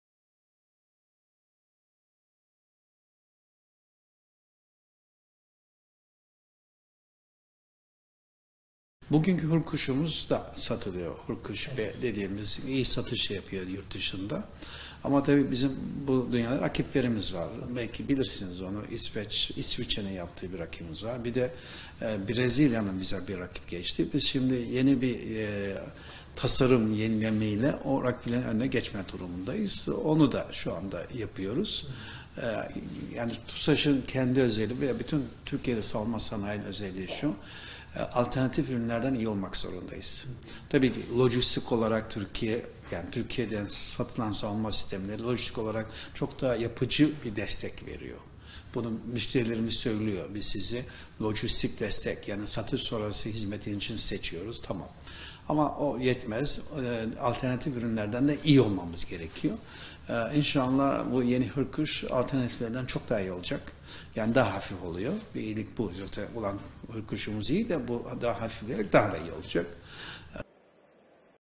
Yeni Hürkuş daha rekabetçi olacak
Genel Müdürü
• Hürkuş ihracatı başladı
• Tasarım yenilemeyle rakiplerin önüne geçme durumundayız, onu yapıyoruz
• Hürkuş'umuz iyi ama yenisi hafifleyerek daha iyi olacak